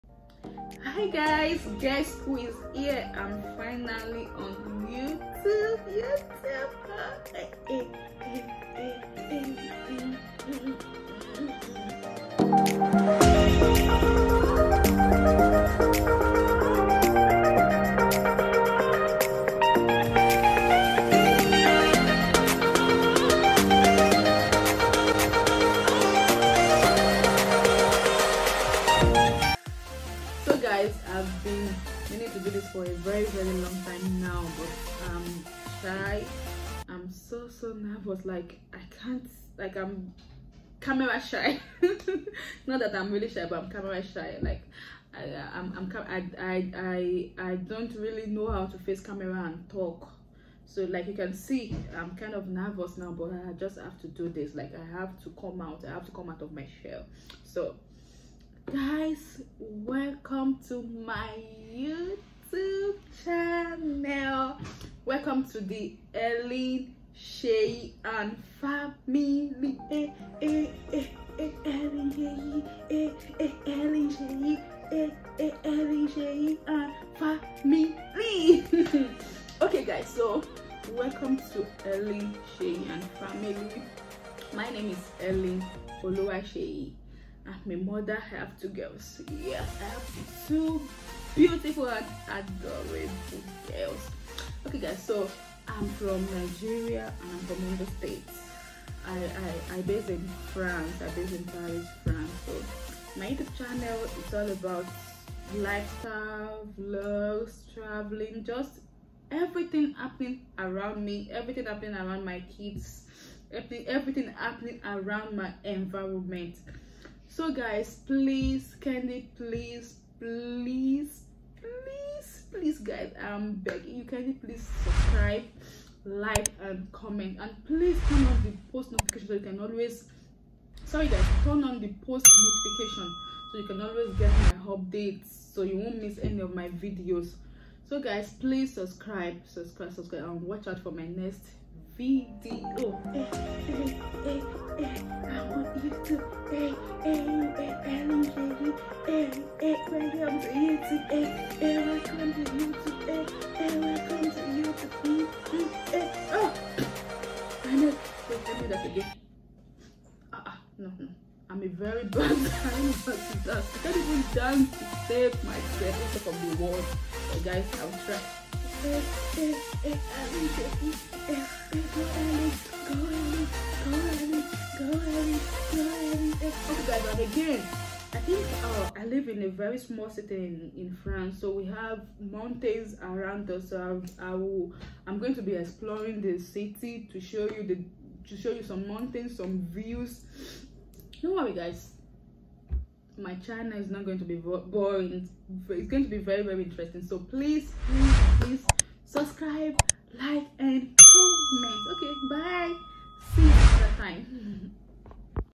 WELCOME TO MY CHANNEL | NEW YOUTUBER TAG
Hi Everyone! This channel is all about Lifestyle, Motherhood , Vlog, Diy, Food, Educative Topics & Talks and I also bring you behind the scenes to my life so do stay tuned and subscribe to my channel for all things HelenSeyi and Family.